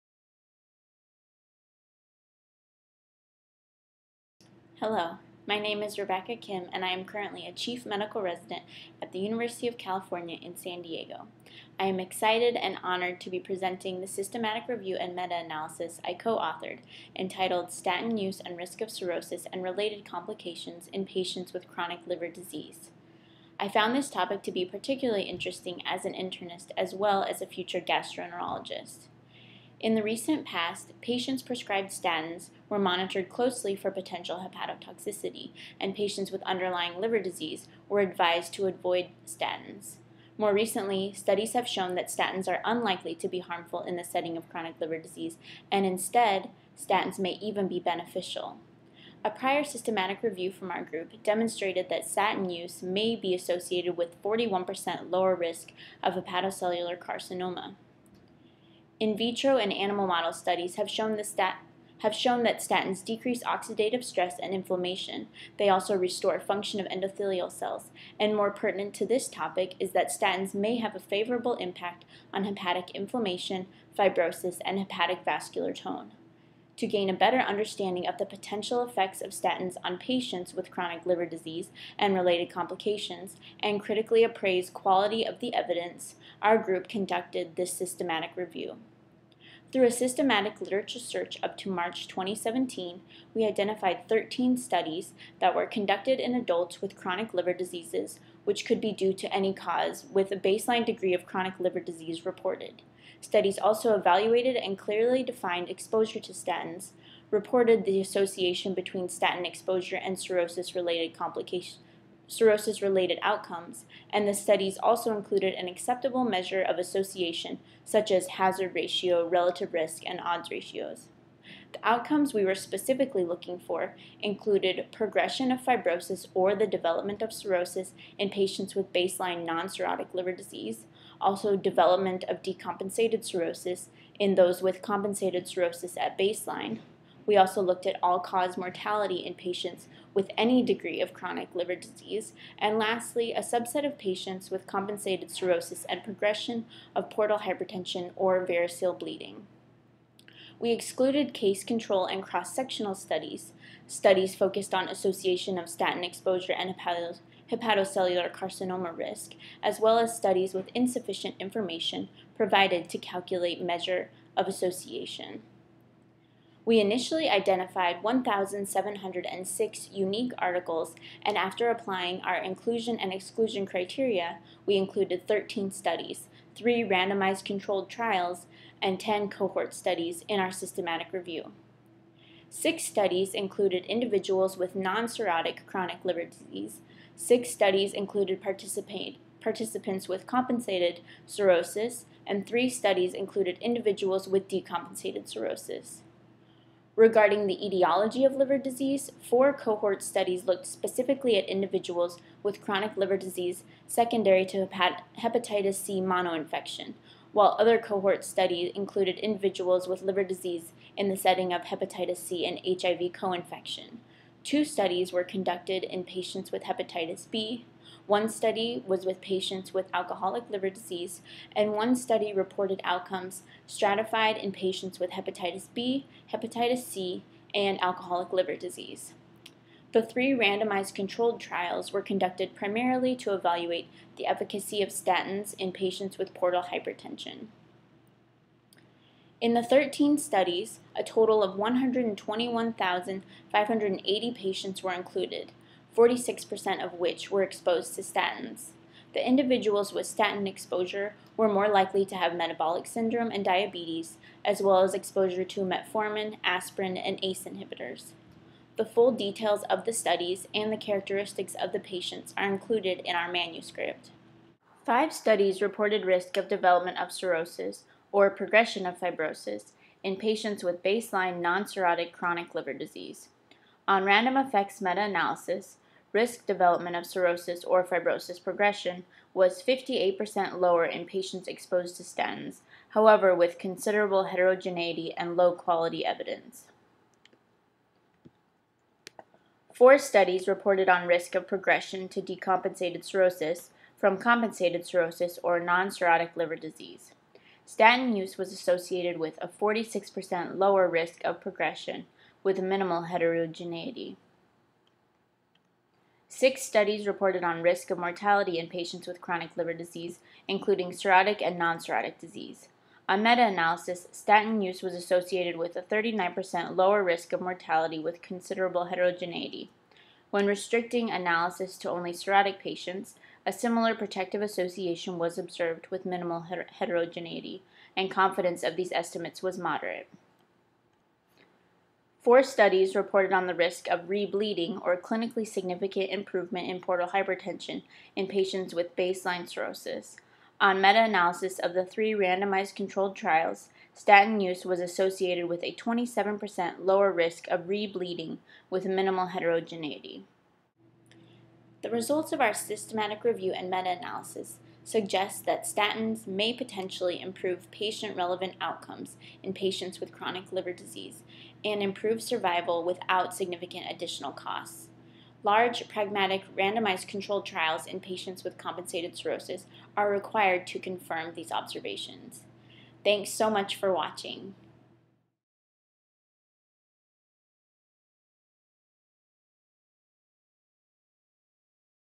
Statins and Outcomes in Chronic Liver Diseases
Dr. Rebecca Kim discusses her manuscript "Statin Use and Risk of Cirrhosis and Related Complications in Patients With Chronic Liver Diseases: A Systematic Review and Meta-analysis" for the October issue of Clinical Gastroenterology and Hepatology.